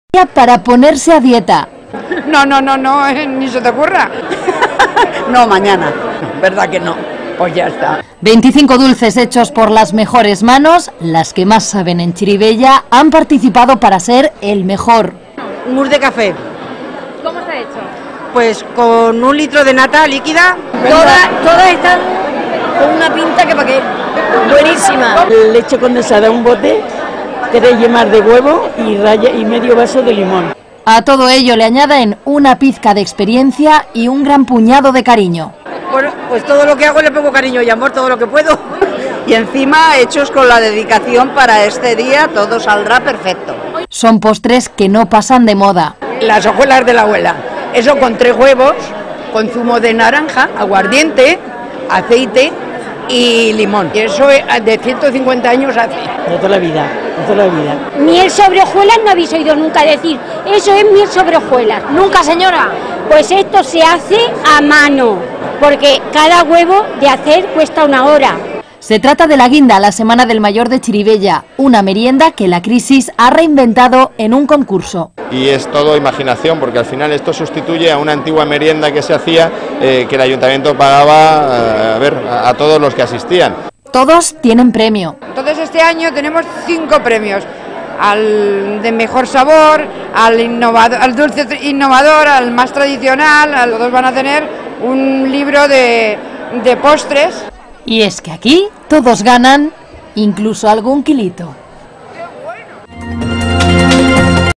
Concurso de postres tradicionales en Gran Turia
Concurso de postres tradicionales en Gran Turia, una actividad de 'La Semana del Mayor 2013'.

Celebración de 'La Semana del Mayor 2013', una iniciativa del Centro Comercial Gran Turia en colaboración con el Ayuntamiento de Xirivella, donde los mayores tuvieron la oportunidad de realizar todo tipo de talleres y actividades culturales.